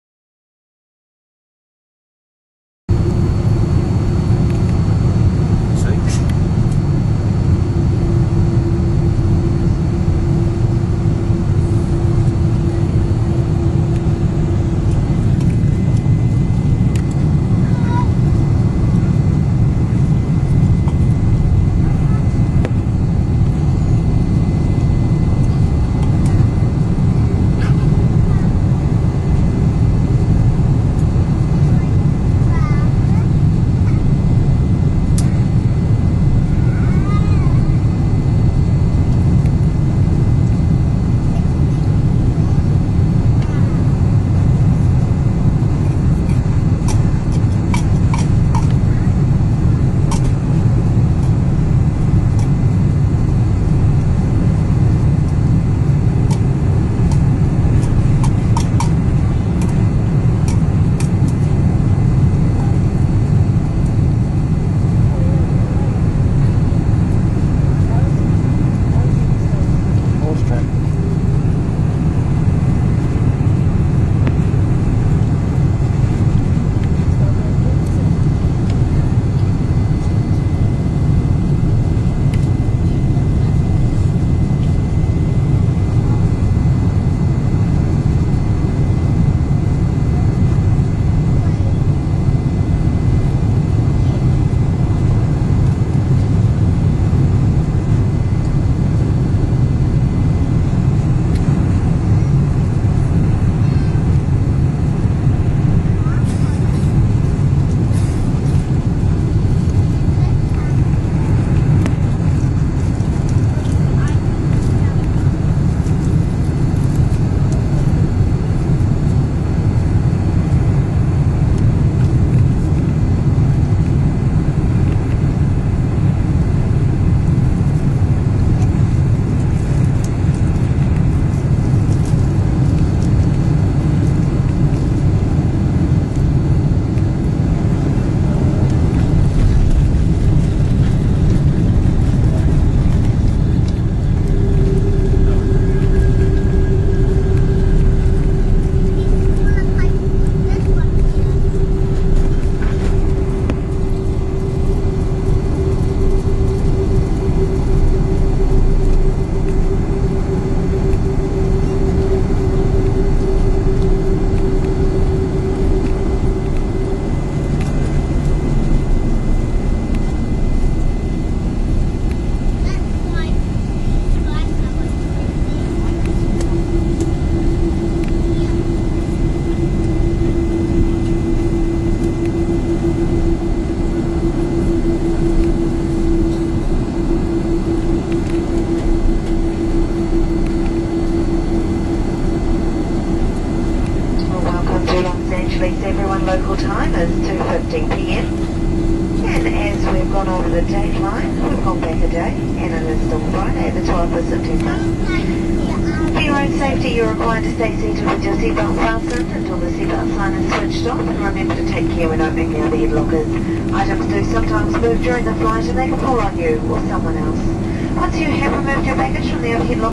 Flying into Los Angeles
Landing in Los Angeles on a smoggy september afternoon. On a Air New Zealand 747